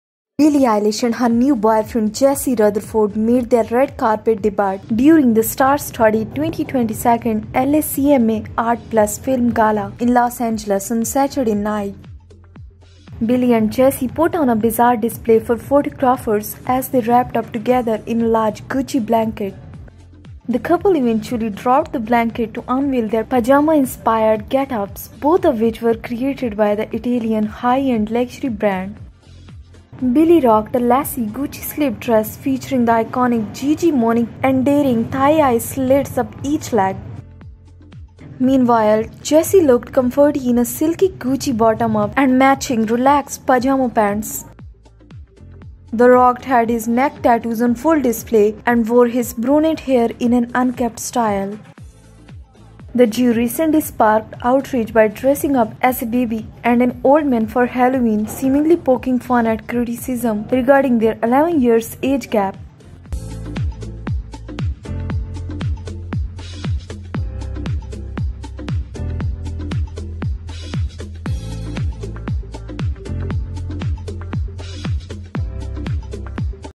Watch Billie Eilish and Jesse Rutherford wrapped in a blanket at 2022 LACMA Art+Film Gala
Billie Eilish and her new boyfriend Jesse Rutherford made their red carpet debut during the star-studded 2022 LACMA Art+Film Gala in Los Angeles on Saturday night.

Billie, 20, and Jesse, 31, put on a bizarre display for photographers as they wrapped up together in a large Gucci blanket.

The duo recently were criticized by dressing up as a baby and an old man for Halloween, seemingly poking fun at criticism regarding their 11 year age-gap.